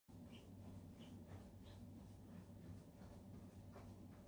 Jog In Place Exercise Demo
This one is for the beginners! Jogging in place is a simple but effective way to get your blood flowing and build a little heat without too much impact.